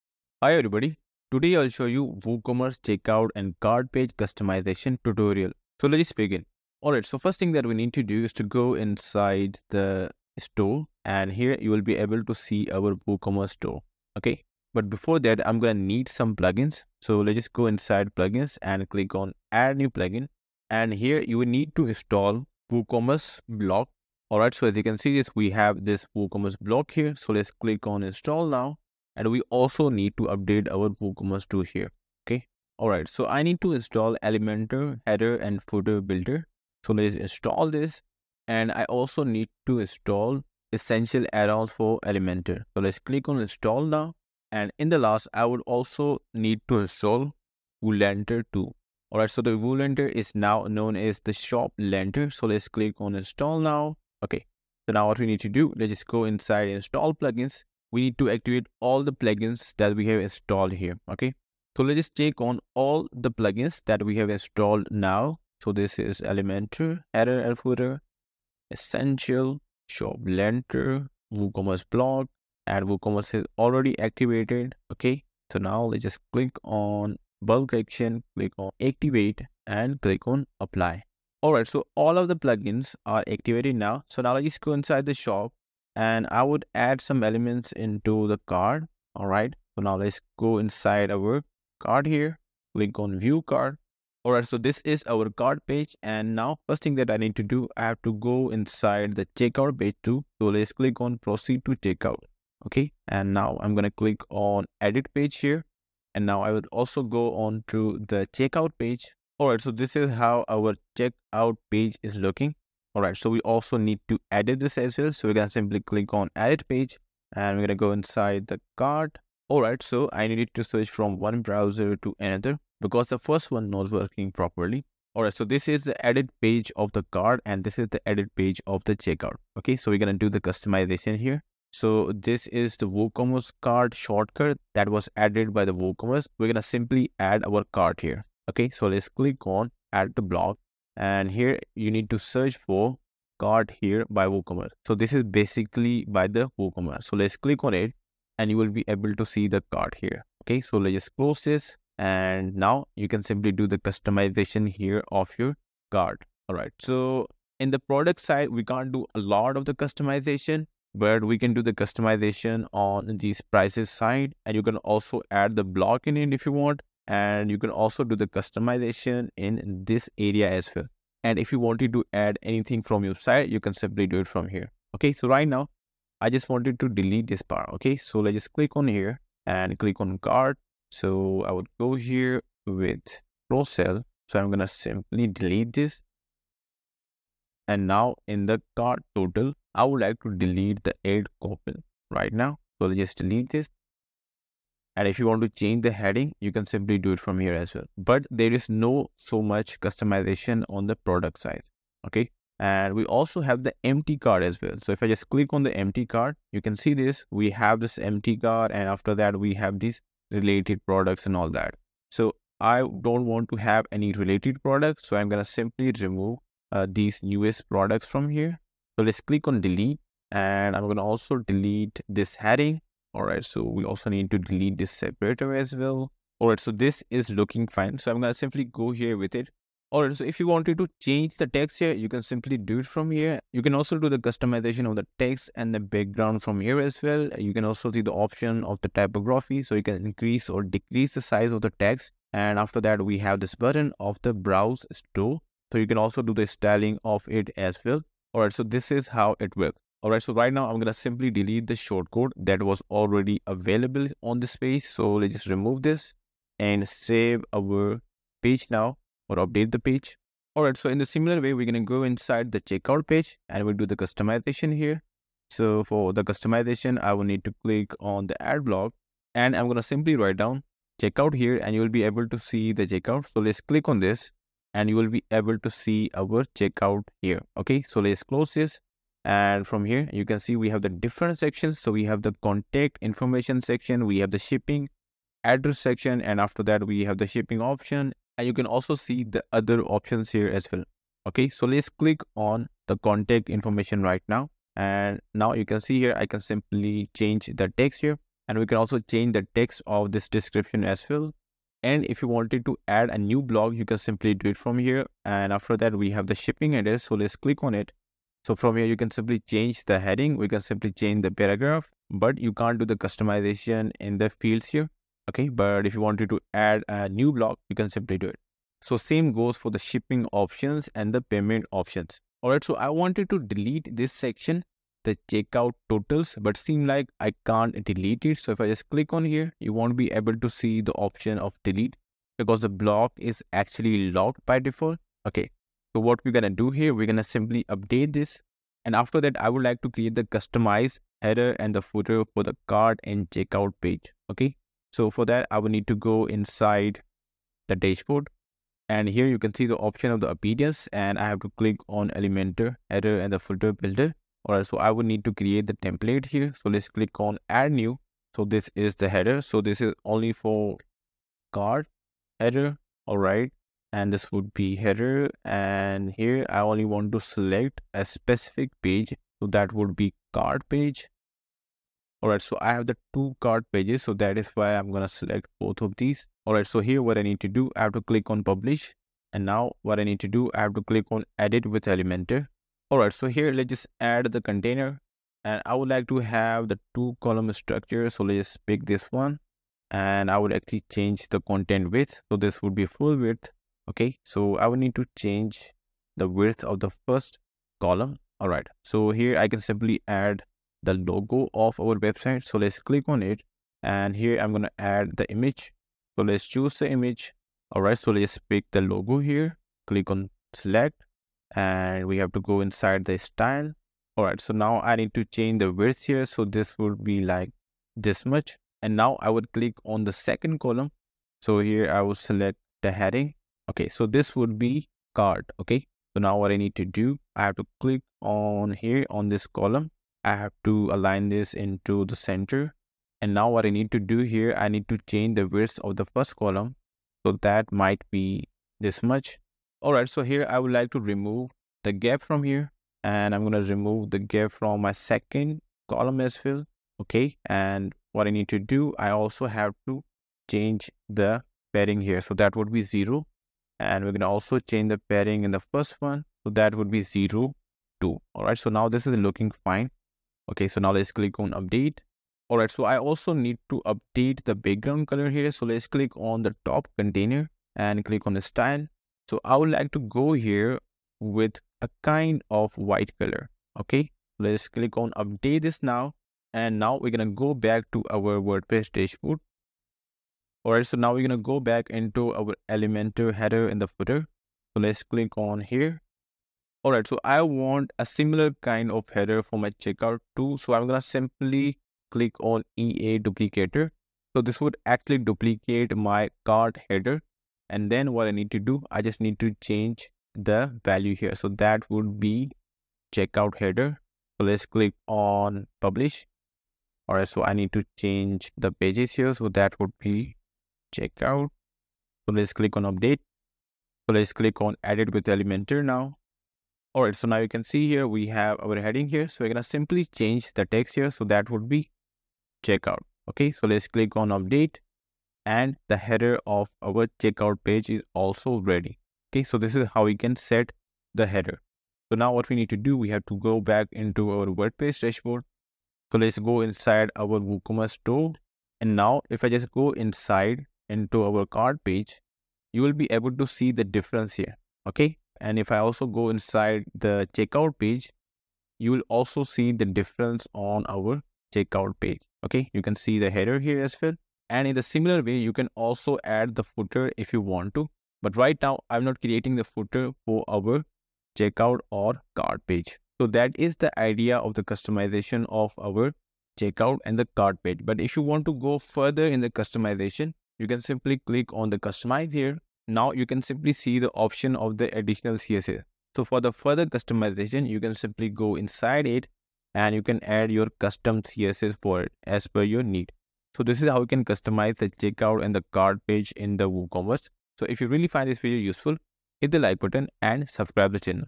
WooCommerce Checkout and Cart Page Customization Tutorial (step by step)
In this video I will show you a WooCommerce checkout and cart page customization tutorial.

Do you want to learn how to create cart and checkout page in WooCommerce? Yes? Perfect! Then you've come to the right WooCommerce tutorial.

I will show you how to setup WordPress WooCommerce cart & checkout page and WooCommerce checkout & cart page customization.

This is a step by step WordPress tutorial on how to customize cart page in WooCommerce and how to customize checkout page in WooCommerce.